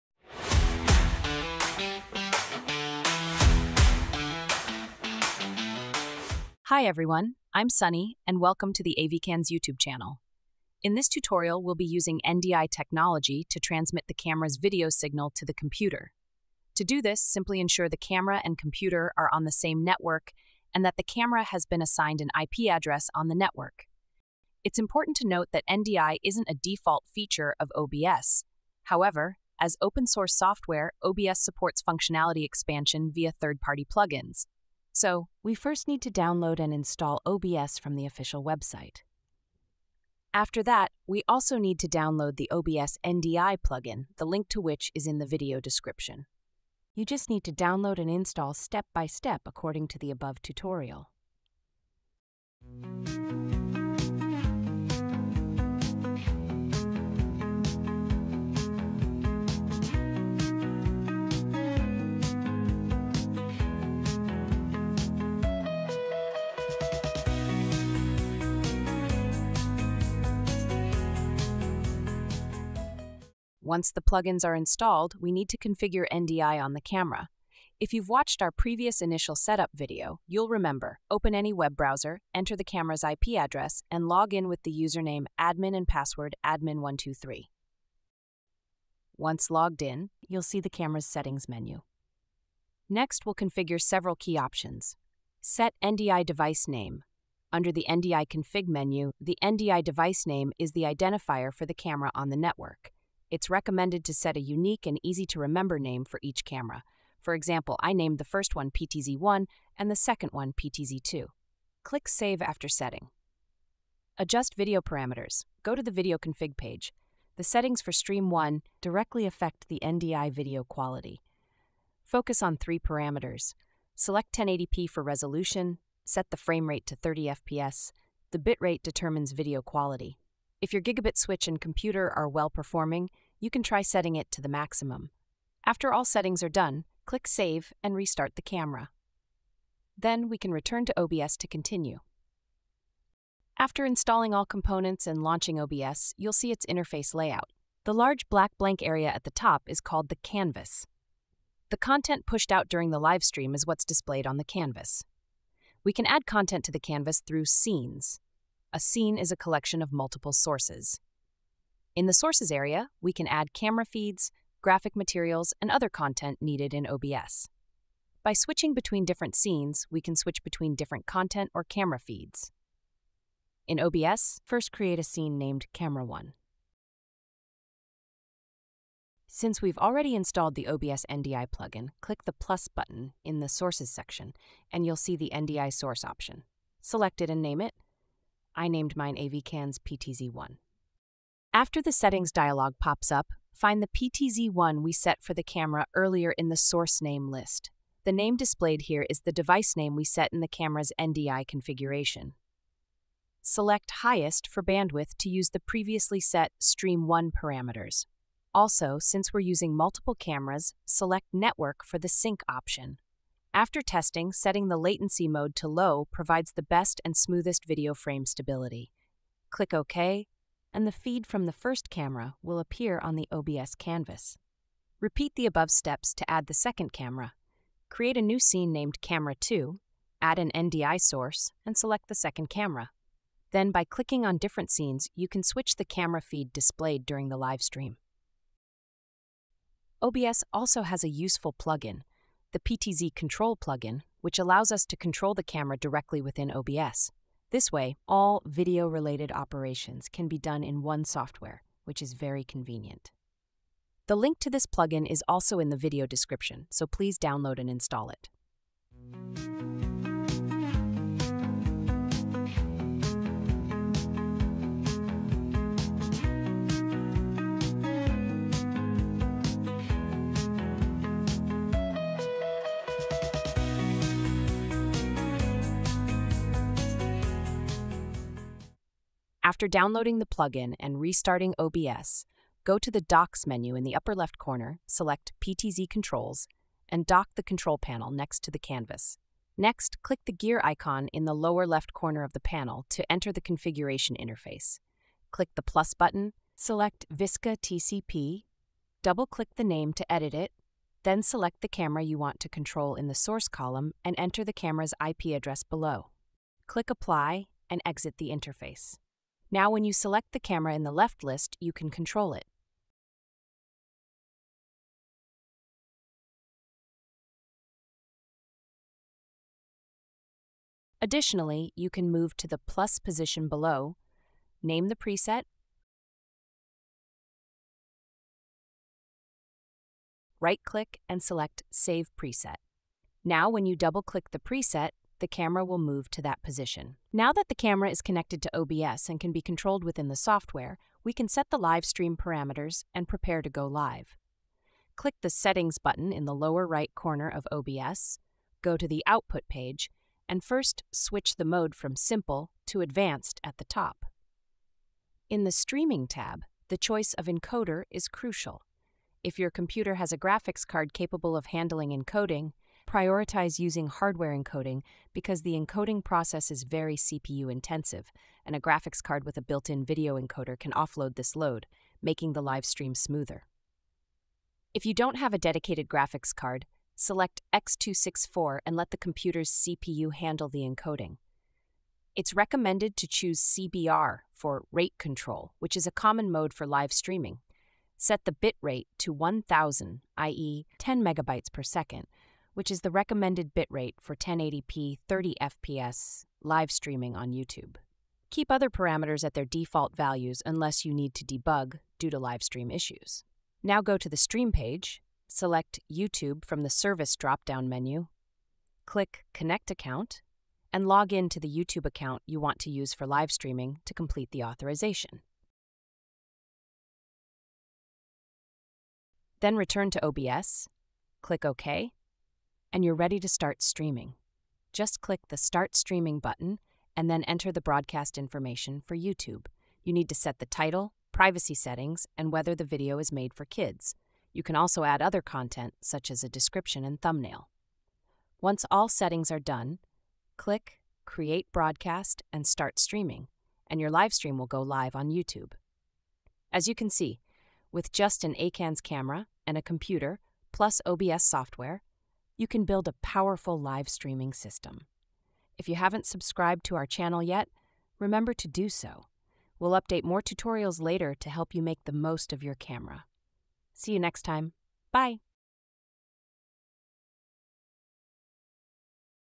How to Live Streaming with OBS and NDI PTZ Cameras? For Beginner 2025 AVKANS
In this tutorial, you will learn how to transmit video signals from an AVKANS camera to a computer using NDI technology, and use OBS to set up and start a YouTube live stream. The full process includes NDI configuration for the camera, installation and setup of OBS plugins, camera control, and more.
Core Tools Required
An AVKANS camera (needs an assigned IP address; ensure it is on the same network as the computer)
A computer (needs internet access; a dedicated graphics card is recommended for hardware encoding)
Software and plugins:
Key Operation Steps
01:15 Camera NDI Configuration:
Enter the camera’s IP address in a web browser, and log in with the username "admin" and password "admin123". Under "NDI Config", set a unique device name (e.g., "ptz1"). In "Video Config", set "Stream 1" to 1080p resolution, 30fps frame rate, and adjust the bitrate to the maximum based on your network performance. Save settings and restart the camera.
OBS & Plugin Setup:
Install OBS and the two plugins. Create a new scene (e.g., "camera 1"),
02:53 How to use the NDI Plugin
04:41 How to use the PTZ Control Plugin
05:45 YouTube Live Stream Settings & Go Live
 In OBS "Settings", go to the "Output" tab, switch to "Advanced" mode, and prioritize hardware encoding (select "x264" if no dedicated graphics card is available). Set the bitrate to 10000. In the "Stream" tab, select "YouTube", log in to authorize your account. Click "Start Streaming", then enter live stream details (title, privacy settings, etc.) on YouTube. Click "Create Broadcast and Start Streaming" to go live.

Camera Initial Setup Tutorial: [Insert link to previous initial setup video here]